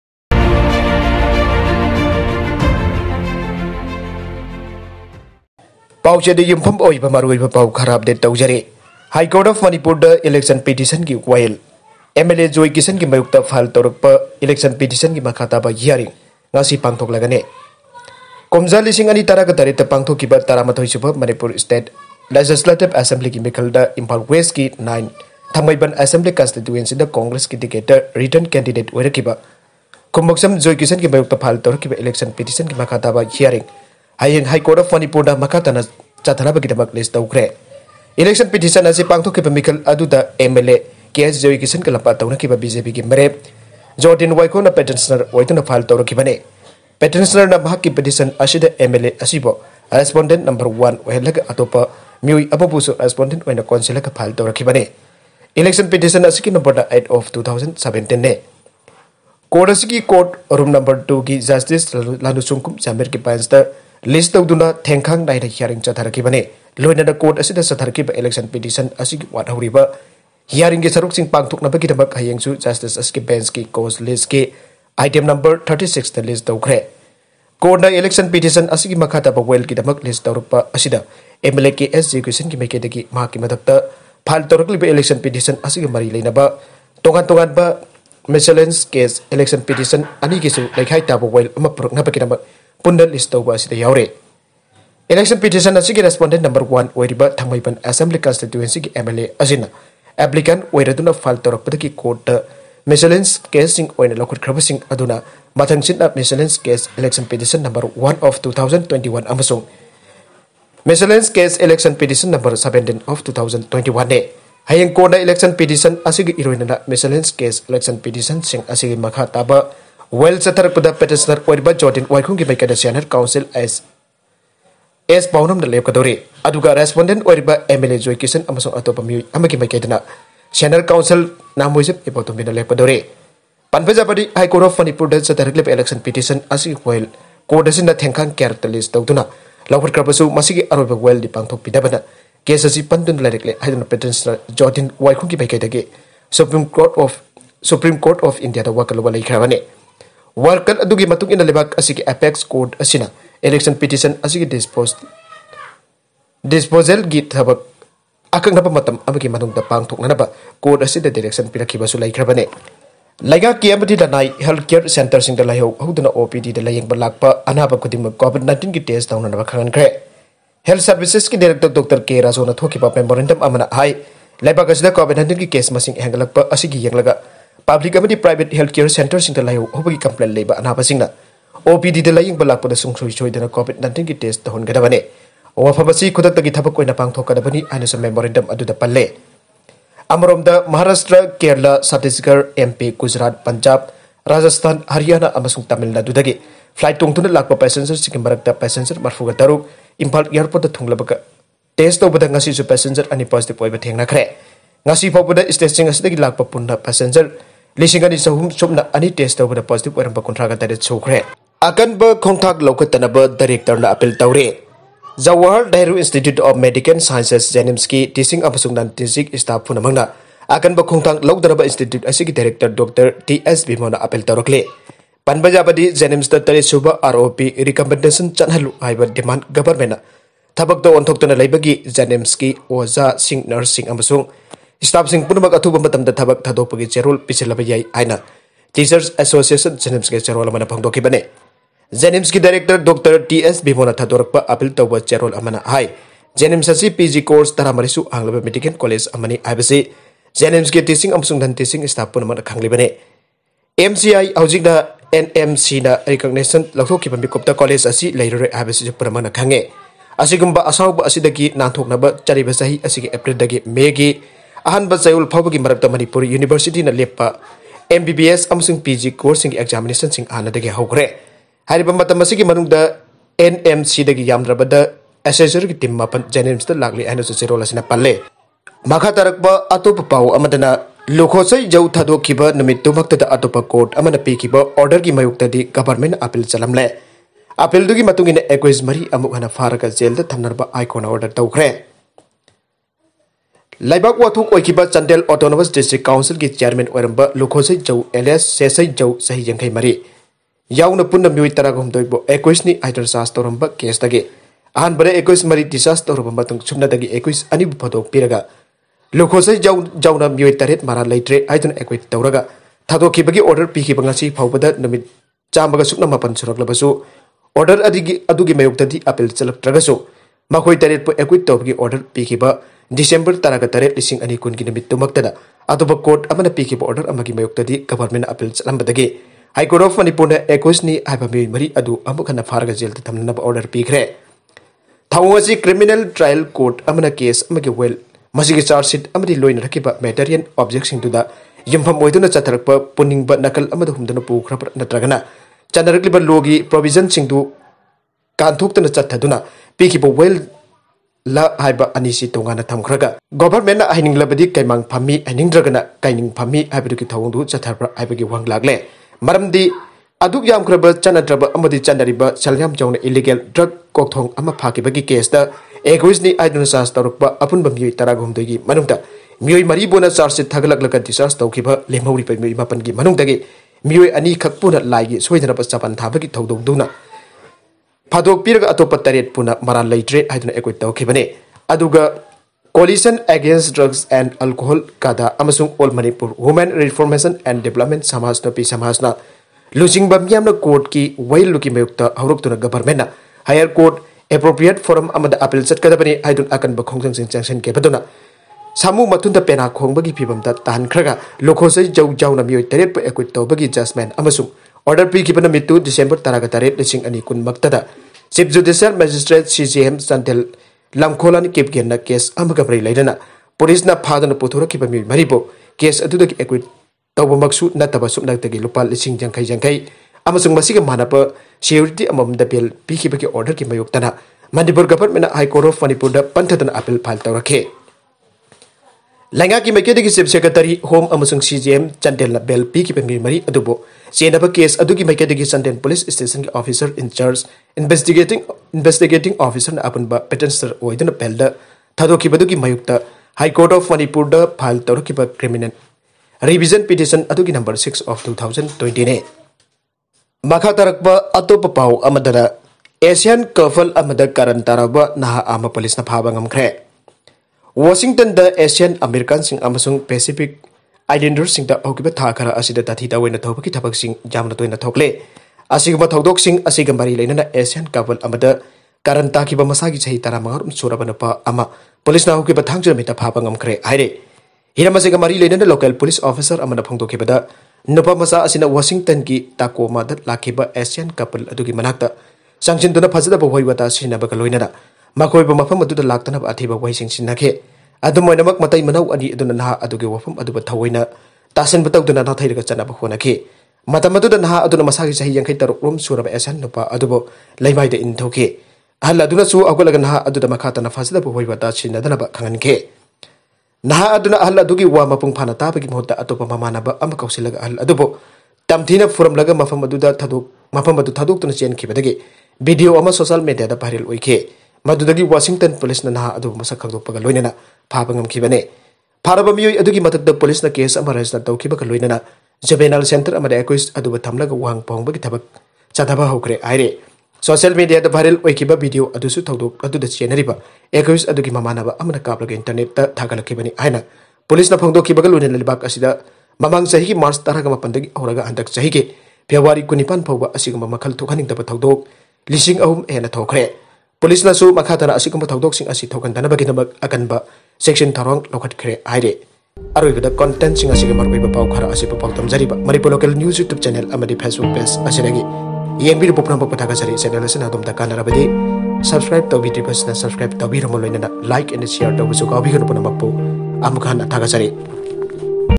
Matei Manao 2 da fajadaba wahei haiduna karan taruba Naha 1 Police na faba ngamkhre
Lukhosei Zou thadokkhiba Numitta Govt.na Appeal chtlamle, Accuse 4 amuk hnna faraga Jail da thmnaba order toukhre ||

Matei Manao 2 da fajadaba wahei haiduna karan taruba Naha 1 Police na faba ngmkhre ||

Laihou houduna OPD da Layengba Lakpa khudingmk C-Test khudktagi  tounanaba khnghnkhre ||

MLA Joykishan gi Mayokta file tourkpa E-pitition makha taba hearing ngasi Pangthoklagani ||